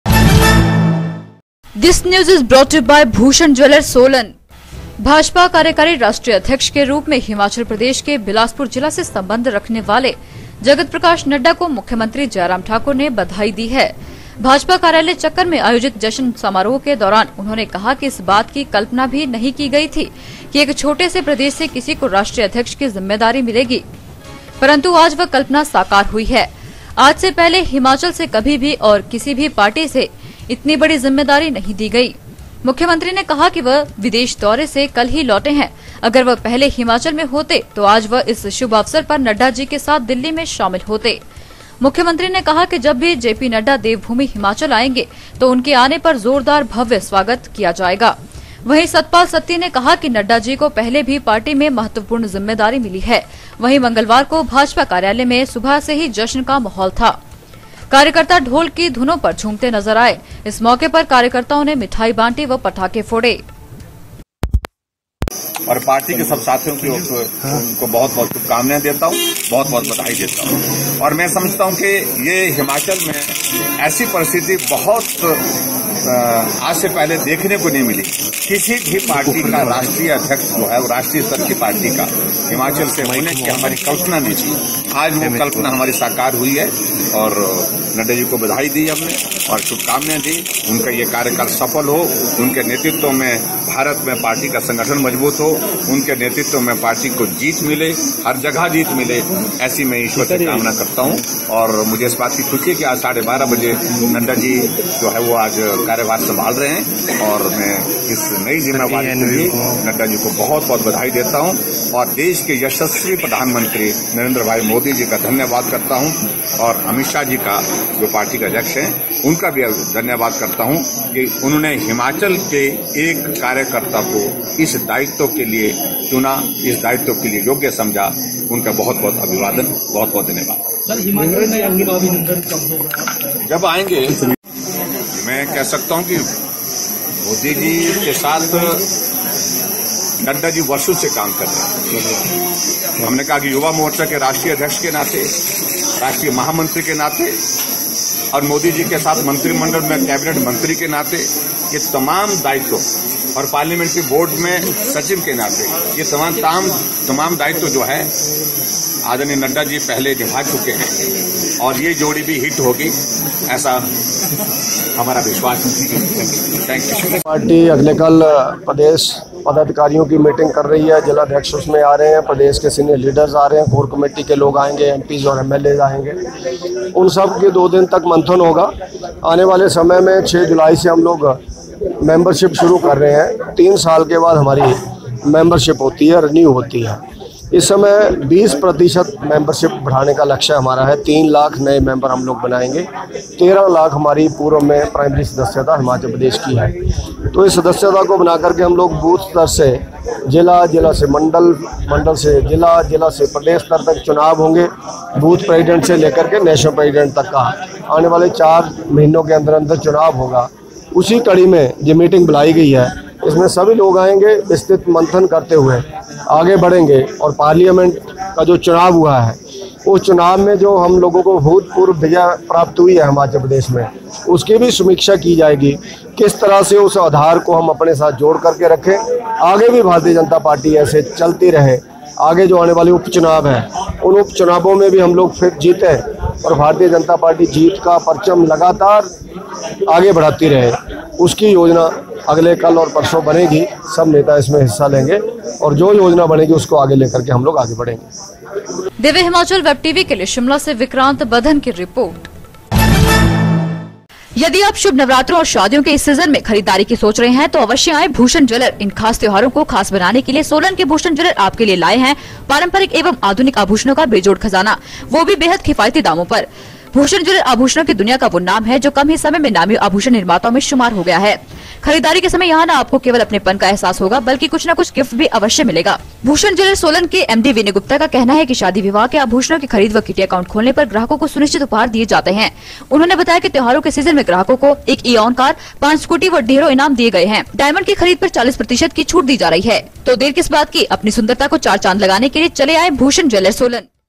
सीएम ने जेपी नड्डा को दी बधाई; बोले, सोचा नहीं था हिमाचल को मिलेगी इतनी बड़ी जिम्मेदारी।
सीएम ने जेपी नड्डा को दी बधाई; बोले, सोचा नहीं था हिमाचल को मिलेगी इतनी बड़ी जिम्मेदारी। दिव्य हिमाचल वेब टीवी